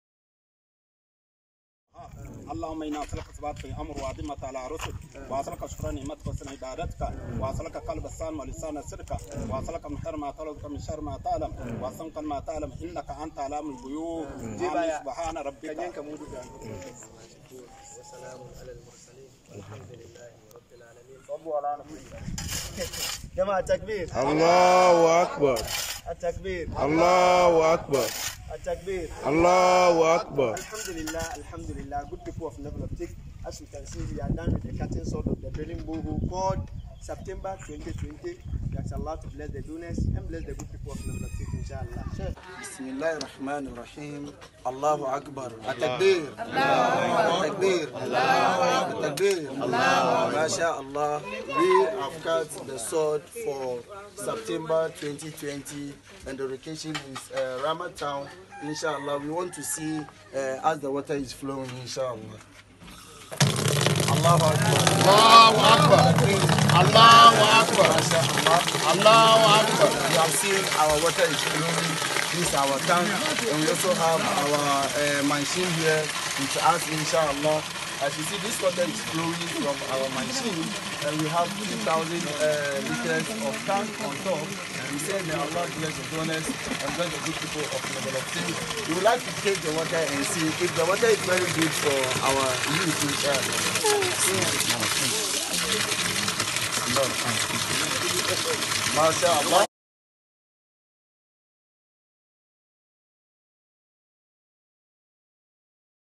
Le puits du mois de septembre 2020 ! Ghana 🇬🇭
Le puits du mois de septembre 2020 !

Le puits se nomme « septembre 2020"

C’est un forage qui se trouve au Ghana 🇬🇭, et qui apportera l’eau à plus de 1000 personnes !

Merci à tous les participants !